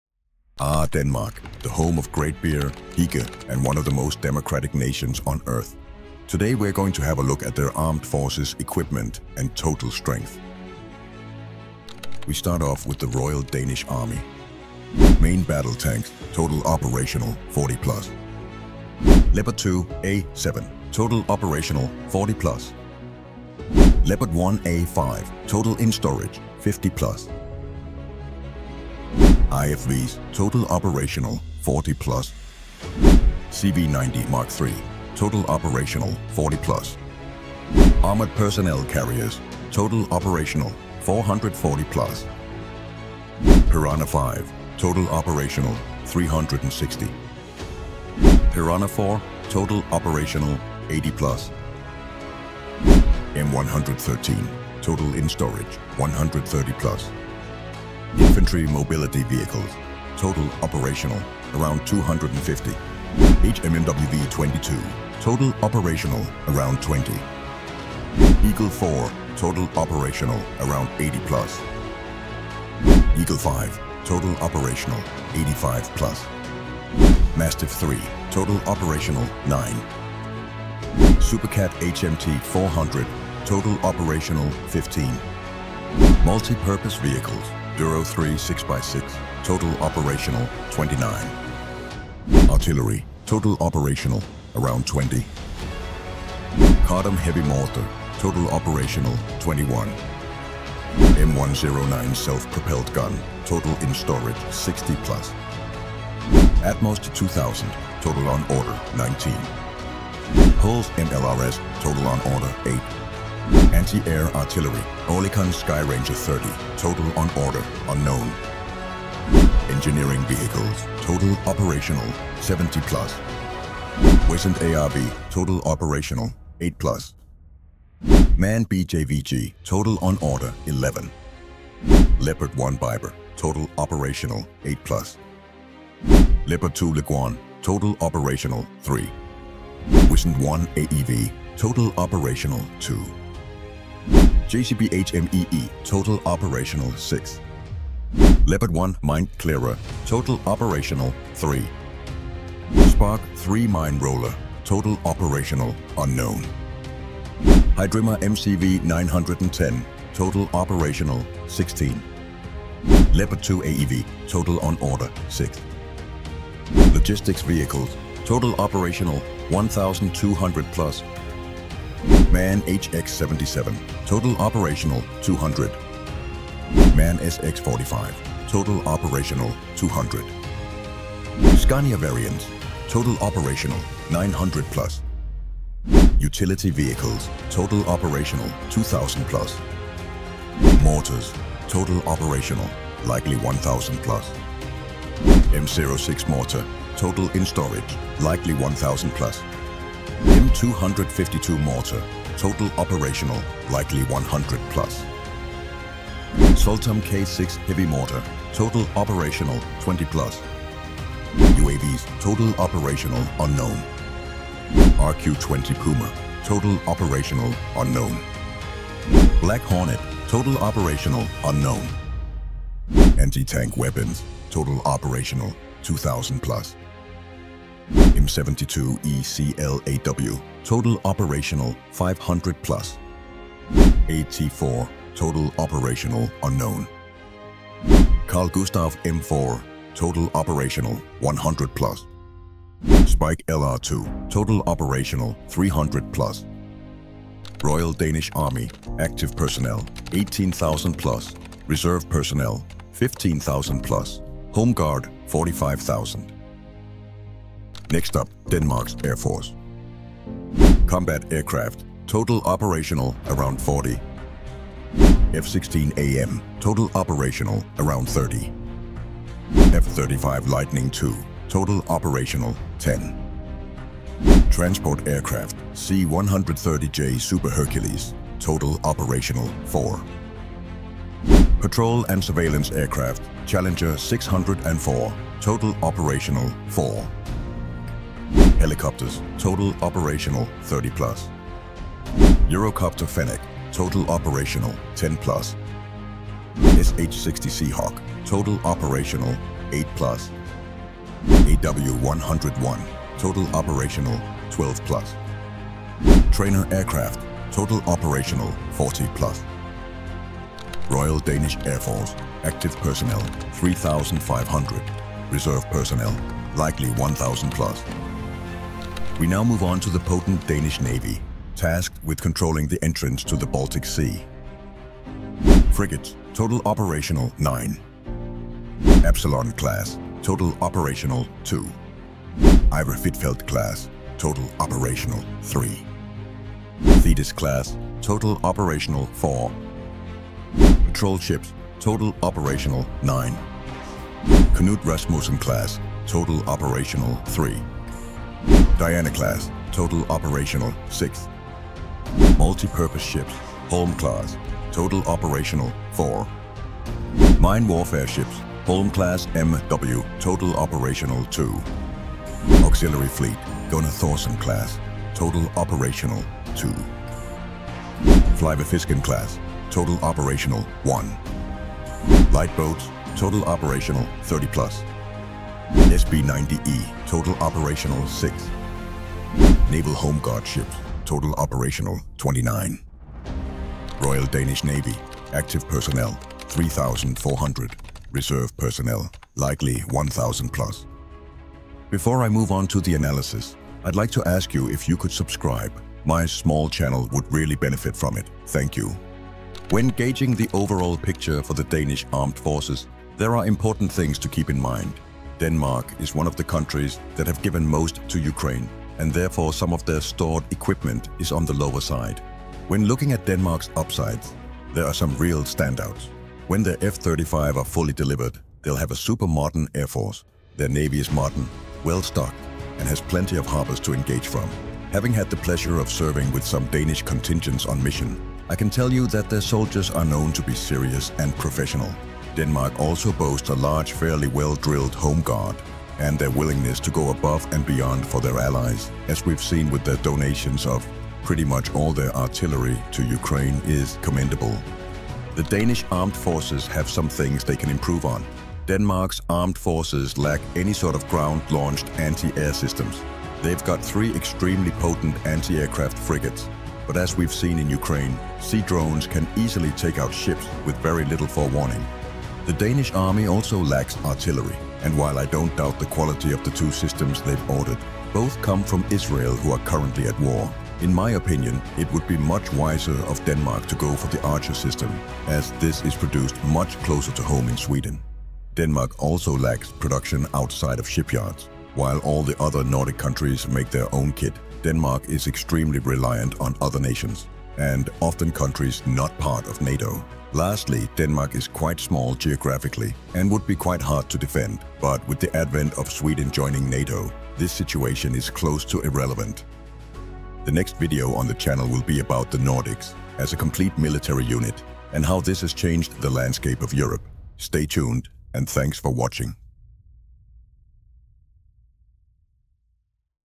Danish Armed Forces | Operational Quantities of Equipment 2024 | How strong is Denmark's defence?
In this review of Denmark's armed forces, the Danish Defence - or in Danish det Danske Forsvaret, we look at the quantities of both manpower and equipment. We also look at the operational quantities of equipment, because it is often less than the actual listed amounts. Please bear in mind that this list is not exhaustive as armies often do not disclose all of their gear.

The Danish Armed Forces - now known as the Danish Defence, is the unified armed forces of the Kingdom of Denmark charged with the defence of Denmark and its self-governing territories Greenland and the Faroe Islands. The military also promote Denmark's wider interests, support international peacekeeping efforts and provide humanitarian aid.

As individual soldier loadouts are often varied, we've not included small arms, mines, uniforms etc.

Any help is greatly appreciated.

00:00 - 00:11 Intro
00:11 - 4:17 Army
4:17 - 5:18 Air Force
5:18 - 6:31 Navy
6:31 - 8:33 Analysis